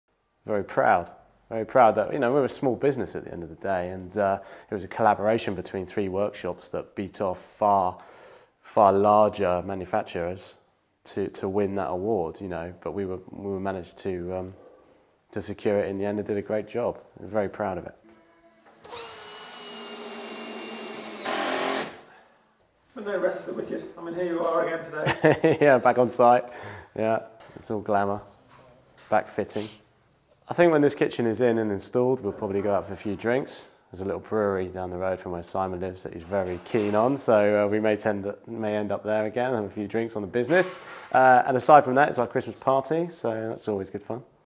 On Winning a Prize in the Wood Awards 2014
So we won the Wood Awards 2014 Bespoke Furniture category with the Makers' Eye / St Hugh's Project and we spent the day after the night before... kitchen-fitting at a new house in Poole. Here's John with his thoughts - and hopes for a celebratory drink - or two - at a brewery, would you believe! We are all feeling very proud.